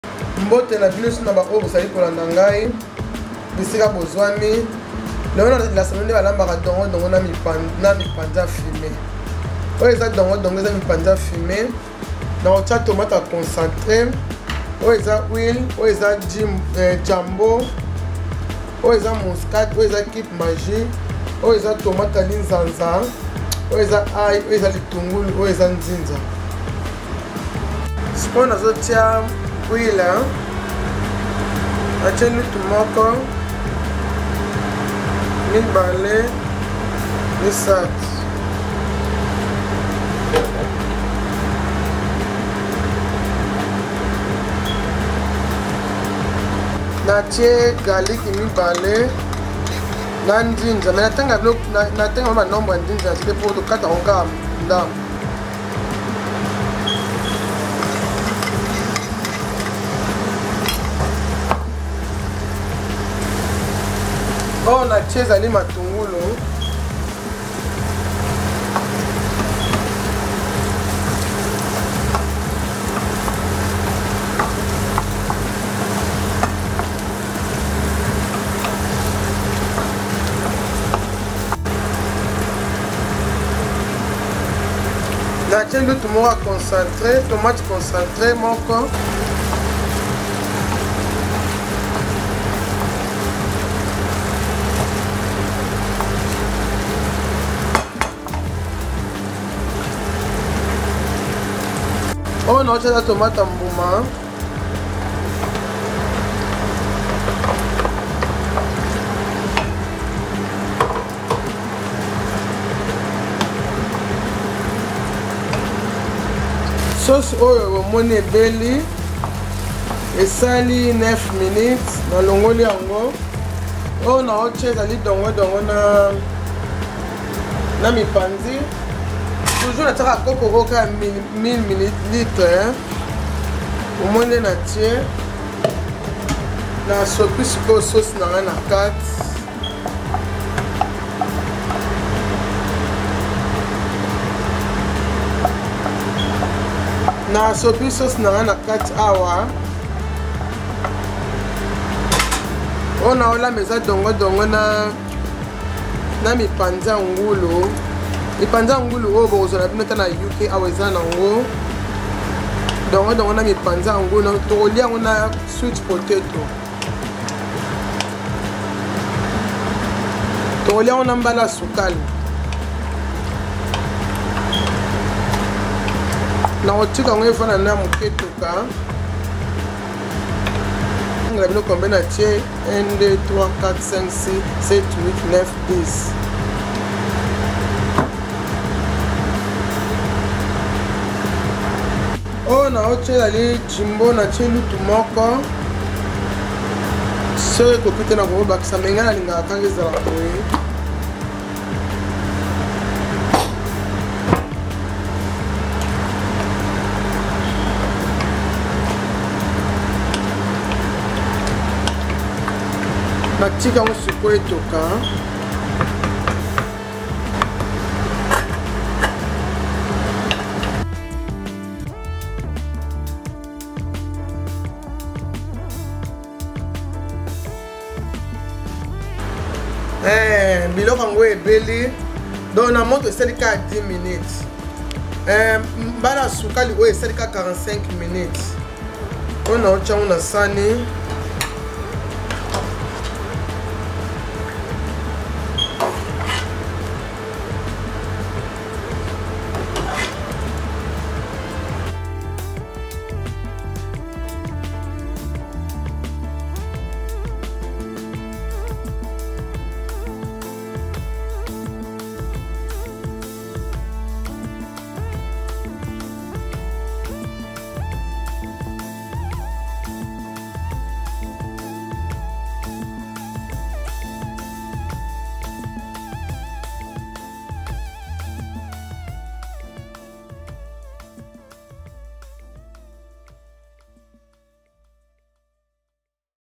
Congolese Food Dongo Dongo (Okra) & Mipanzi Ya Fume (Smoked Beef)
Today's Cuisine Is Dongo Dongo (Okra) & Mipanzi Ya Fume (Smoked Beef).

Dongo Dongo (Okra)
Mipanzi Ya Fume (Smoked Beef)
Sweet Potato
Ginger
Garlic
Onions
Nutmeg
Tomato Puree
Chopped Tomato
Jumbo All Purpose Seasoning

La cuisine d'aujourd'hui est Dongo Dongo (Okra) et Mipanzi Ya Fume (boeuf fumé).

Dongo Dongo (Okra)
Mipanzi Ya Fume (Boeuf fumé)
Patate douce
Gingembre
Ail
Oignons
Noix de muscade
Purée de tomates
Tomate hachée
Assaisonnement tout usage Jumbo

congolese food
congolese cuisine
african food
african cuisine
okra recipe
smoked beef recipe